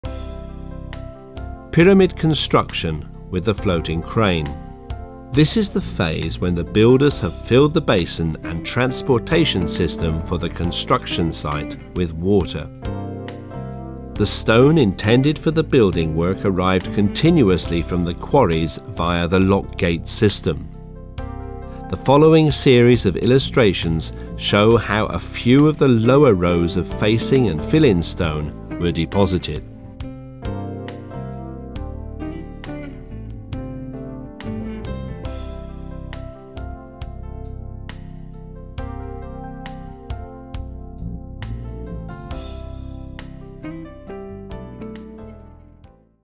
The initial stages of pyramid construction
Chapter 11 / Animation 1
From the book of Pyramids on Water, Floating Stones by Istán Sörös